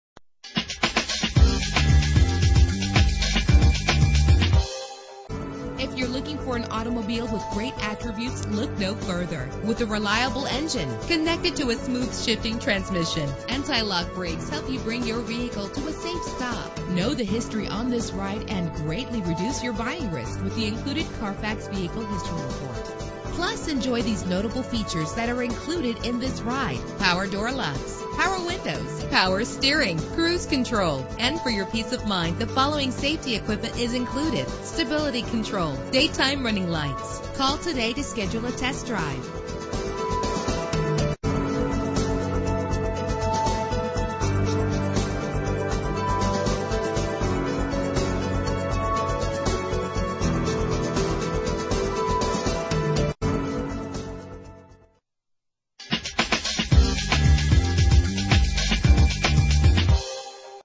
2008 BMW 528i - Ft. Lauderdale FL
Year: 2008
Make: BMW
Model: 528i
Trim: Sedan 4D
Engine: 6 cylinder 3.0 liter
Transmission:
Color: Titanium Silver Metallic
Mileage: 25696
Address:
1400 South Federal Hwy